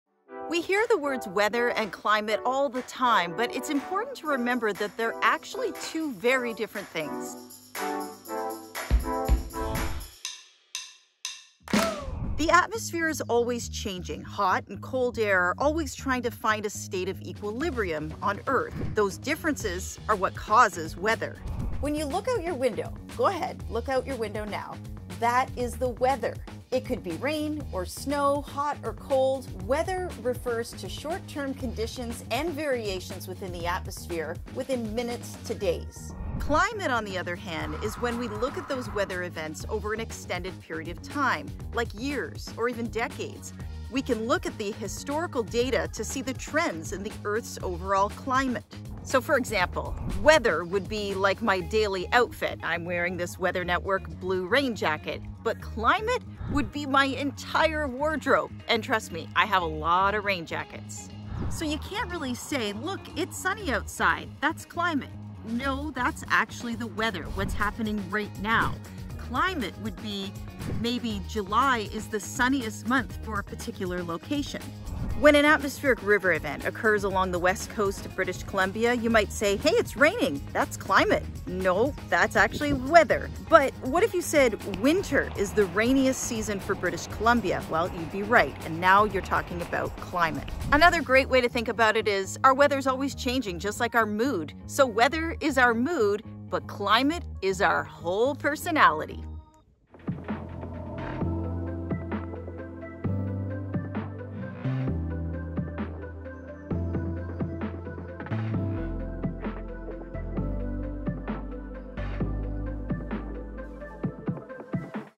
Weather Vs. Climate: What's The Difference?
We hear the words weather and climate all the time. But it's important to understand that these are two very different things. The Weather Network meteorologist Jaclyn Whittal explains how to differentiate the two.